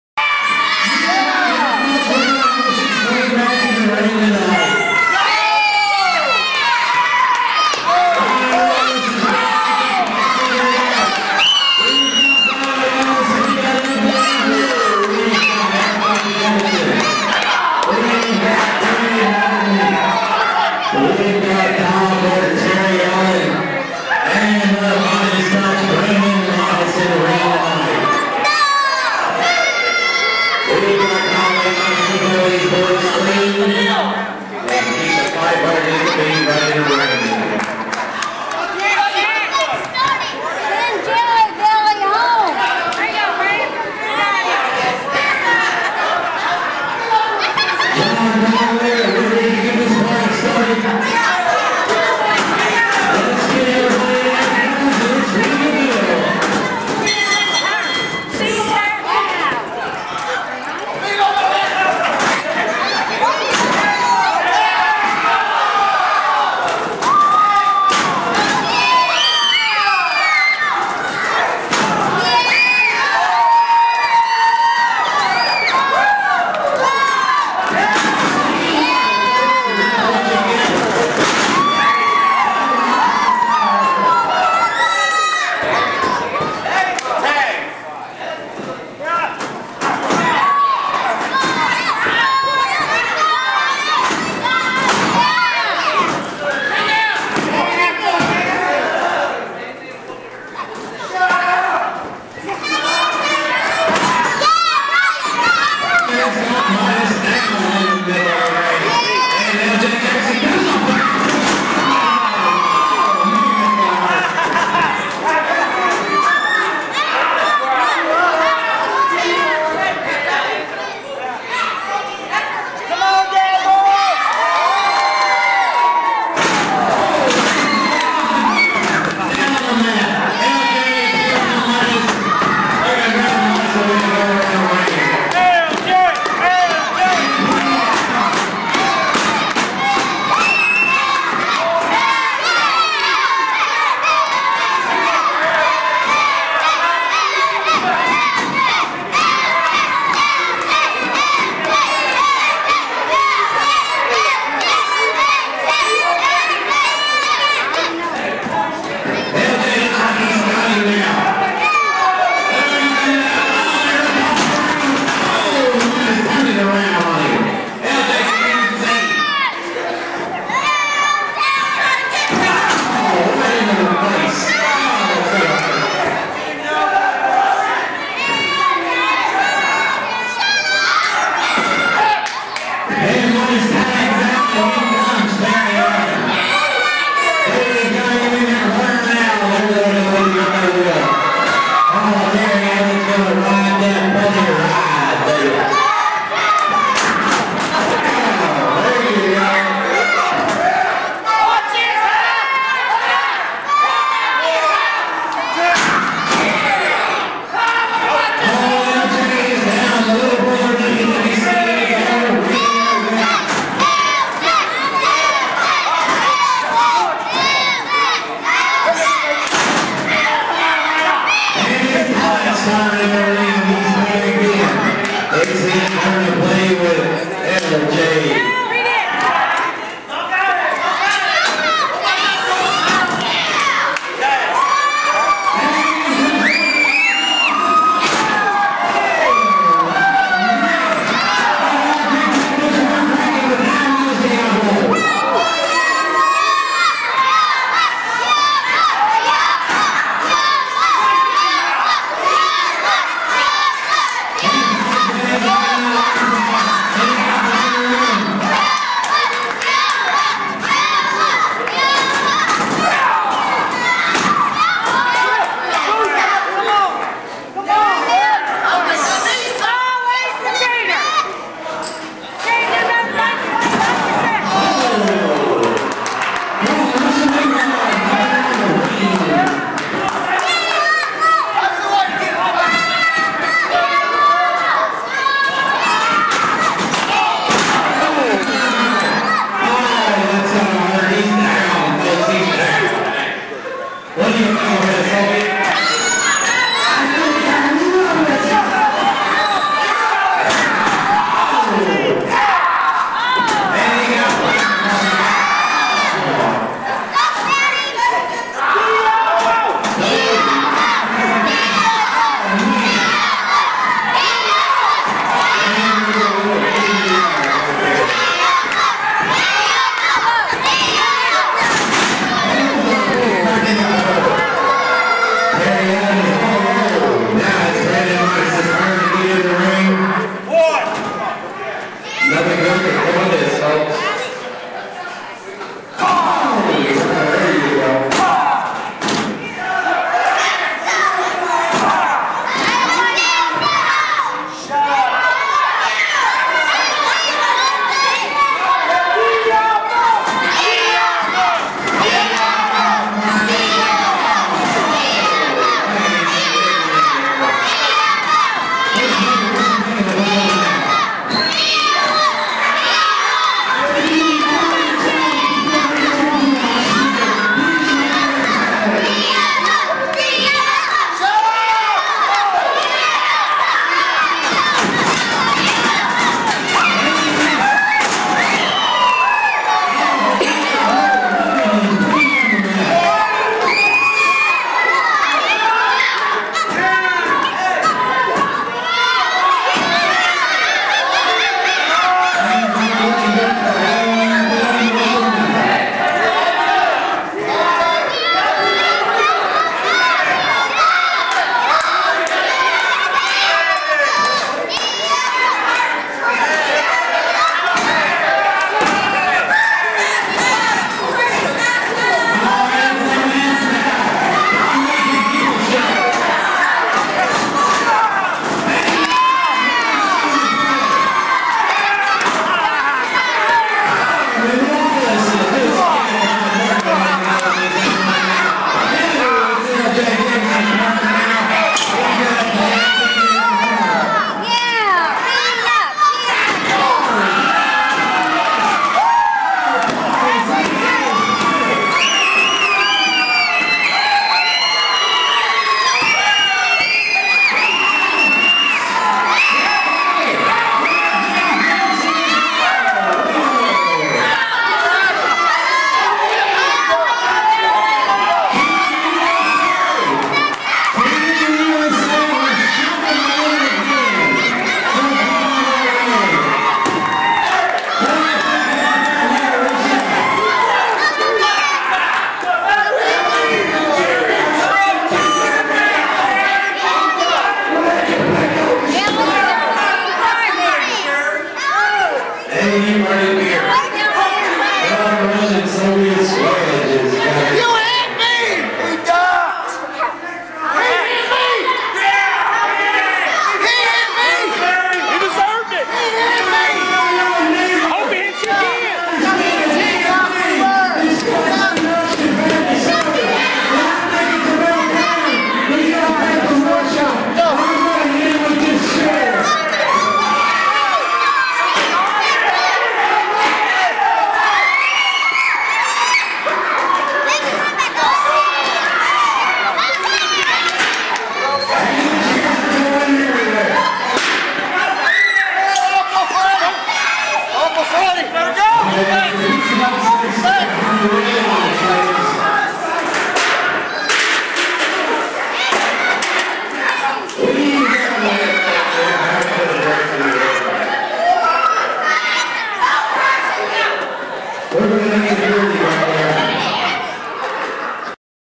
Diablo, LJ & Brian Lawless vs Todd Morton,Brandon Midas & Terry Allen
6 man tag team action after Diablo made the save to help out LJ and Lawless from Todd,Midas and Terry